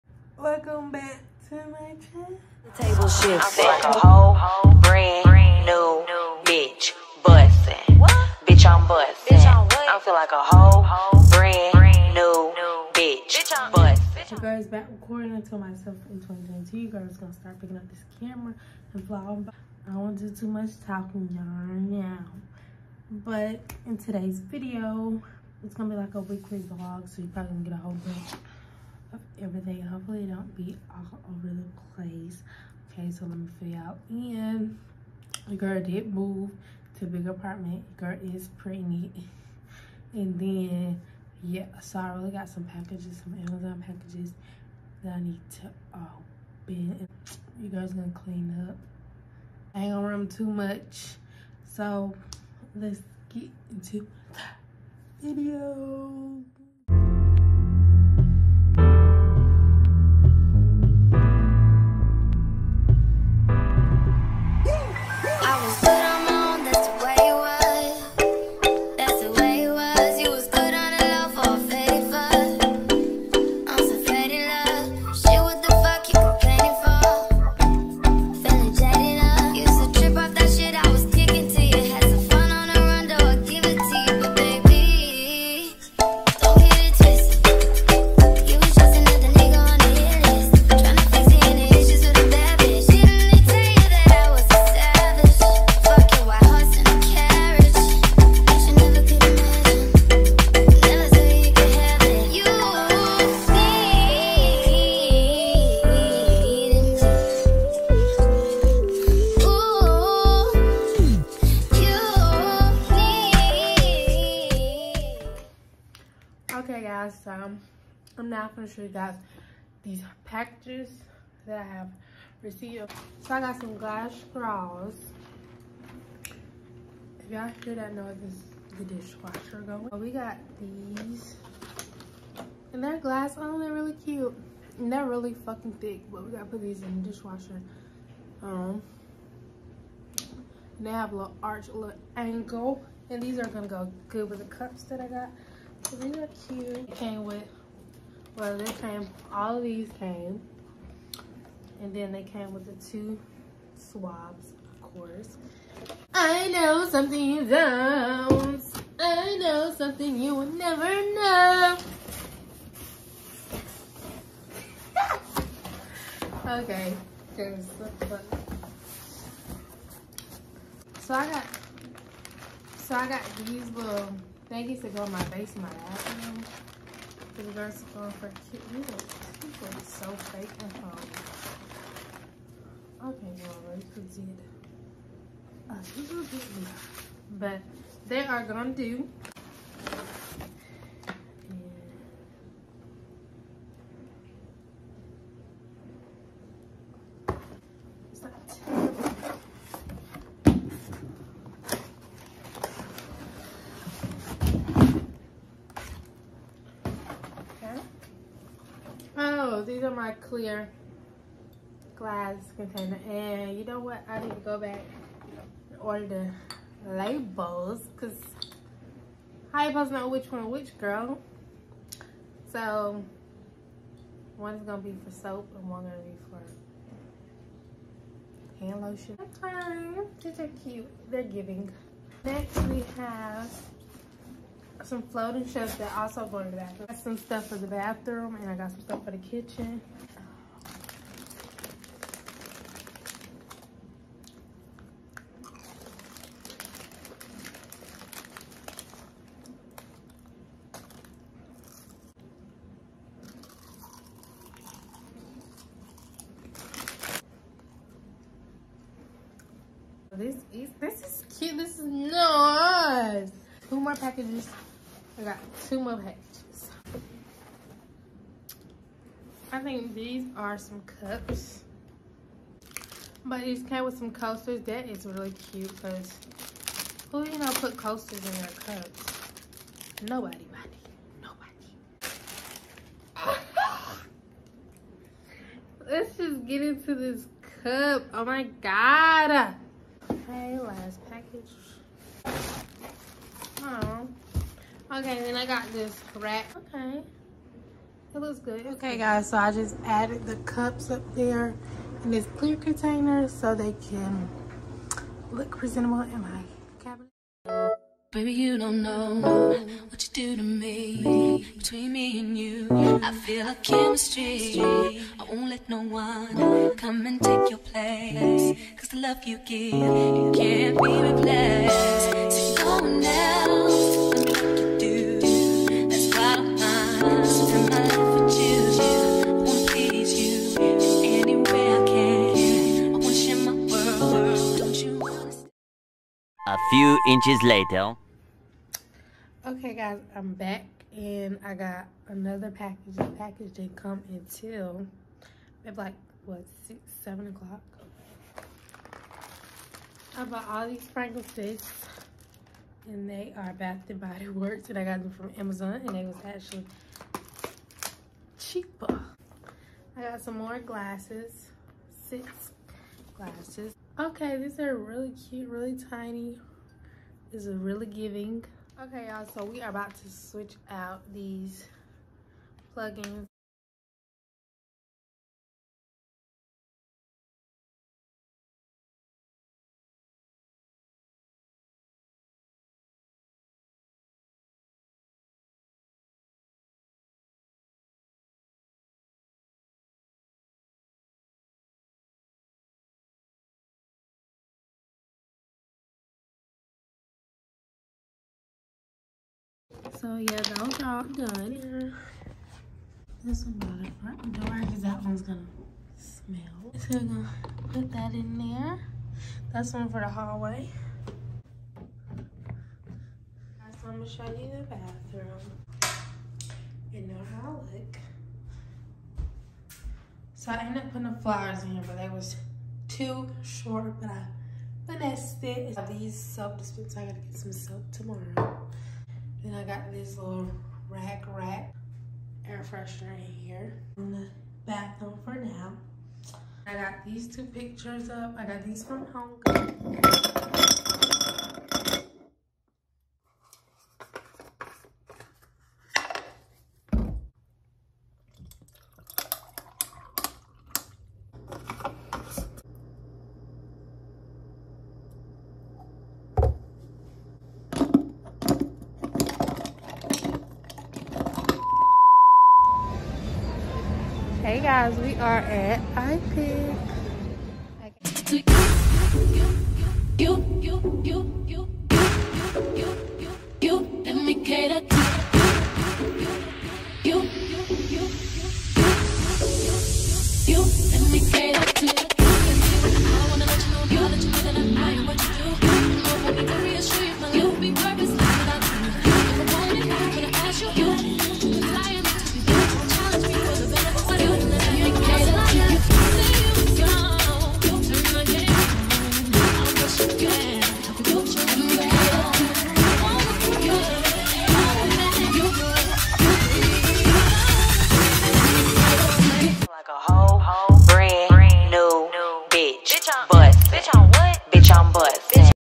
WEEKLY VLOG || unboxing packages, movies cleaning and more
Thank you guys for watching , just a short vlog so i can get back in the grove . Let me know what you guys would like to see and more love guys and thank you !

COMMENTS ‼️
BACK AND BETTER ALL 2022

Don’t forget to hit the nonfiction bell so you get notifyed when I post ✅